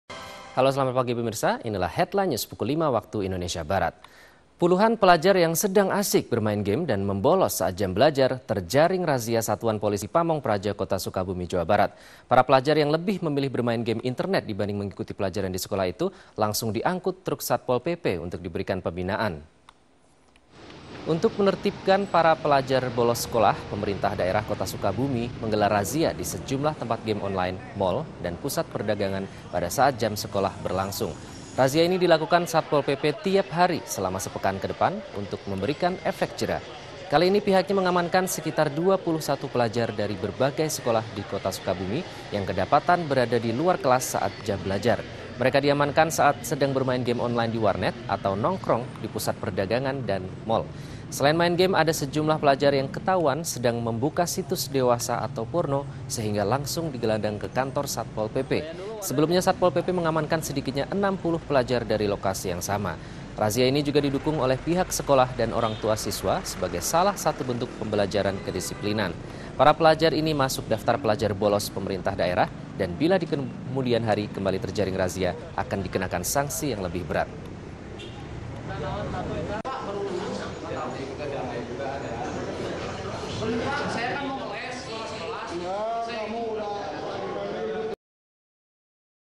Puluhan Pelajar Ditangkap saat Bermain Video Game
Sukabumi: Puluhan pelajar terjaring razia di sejumlah tempat video game online di Kota Sukabumi, Jawa Barat, Jumat (31/5). Mereka ditangkap karena bermain pada saat jam sekolah.

Selain game, beberapa pelajar juga tertangkap basah tengah membuka situs porno. Semua yang terkena razia digelandang ke kantor Satuan Polisi Pamong Praja Kota Sukabumi.

Operasi disiplin ini didukung pihak sekolah dan orang tua siswa. Petugas berencana melakukan hal serupa untuk sepekan ke depan.